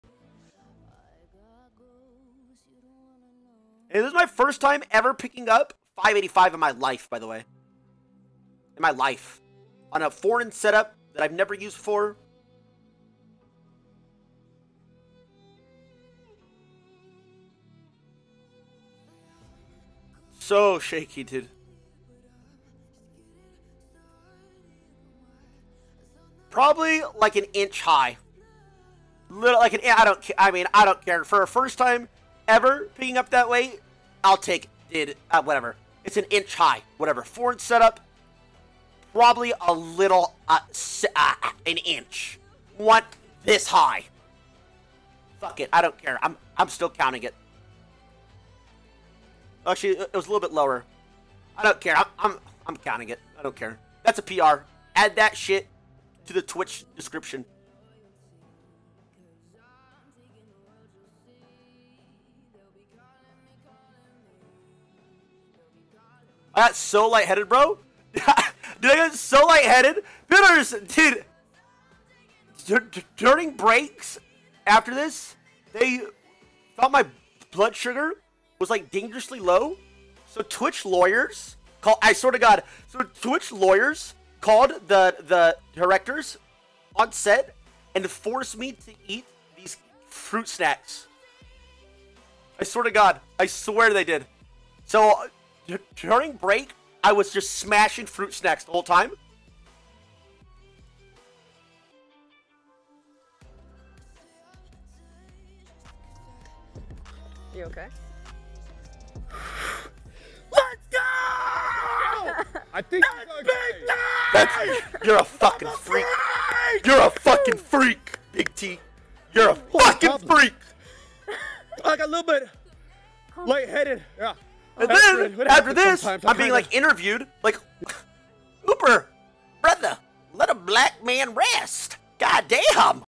Tyler1 Reacts To His INSANE Squat PR - Call From Twitch Lawyers!!!
Tyler watches his squat PR